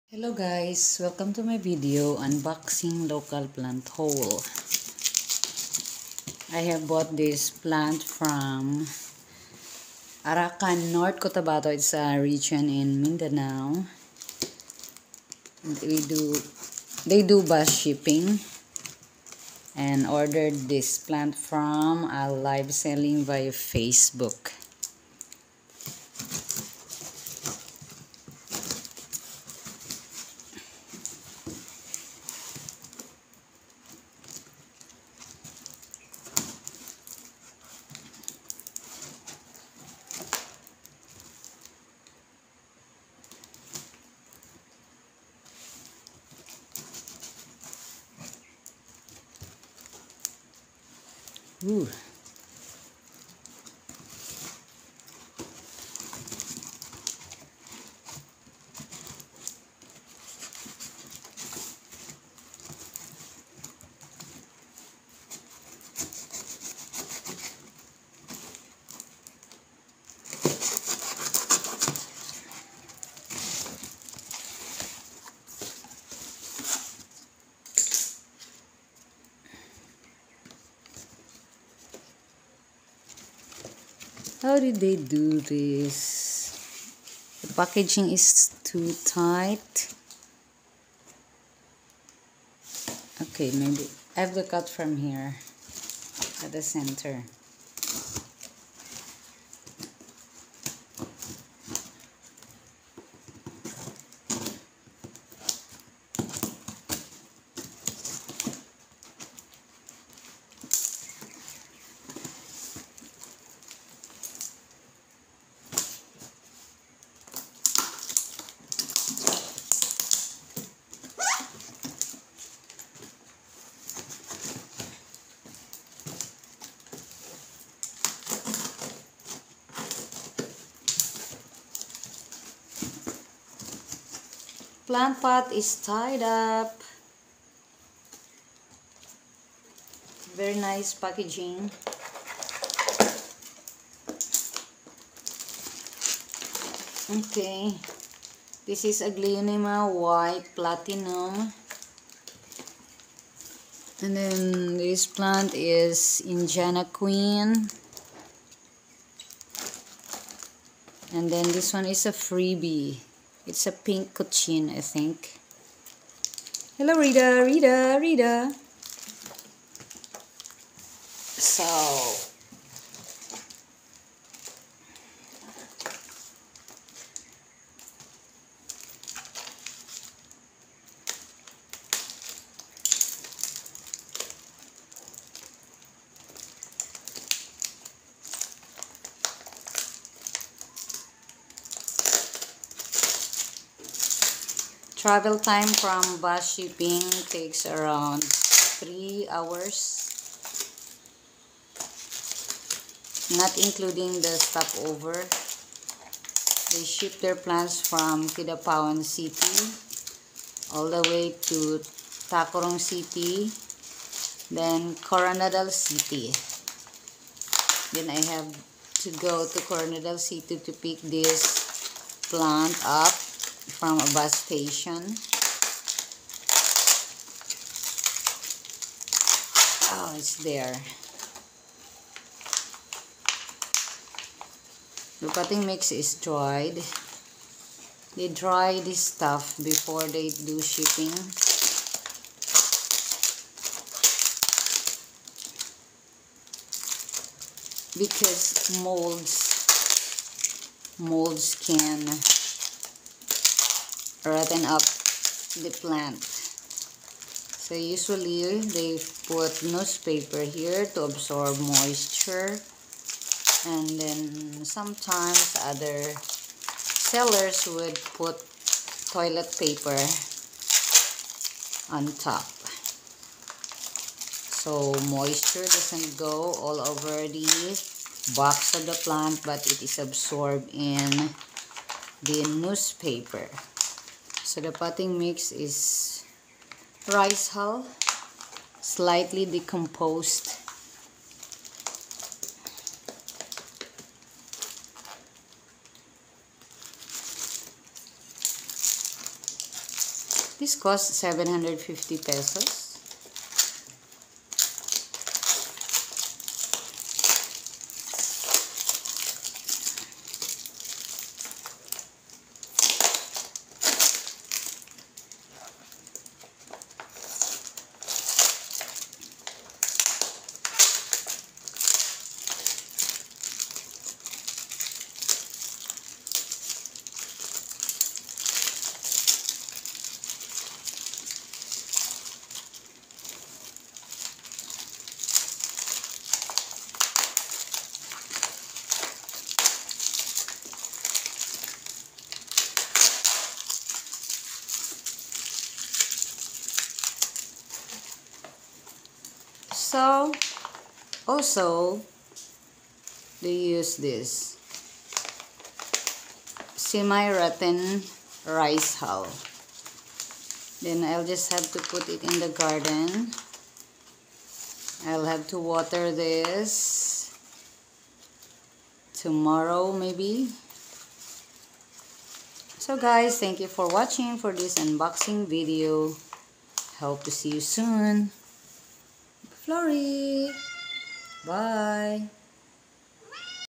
Today's Unboxing of aglaonema plants from a local supplier
Aglaonema white platinum, Indiana Queen and pink Cochin goes in one box!

Best feature of this package is the plant pot is tied down to stay at the bottom of  pot so even if  the box is turned upside down the plant is secure!

Amazing packers at Mary Jean Anarna live selling via Facebook.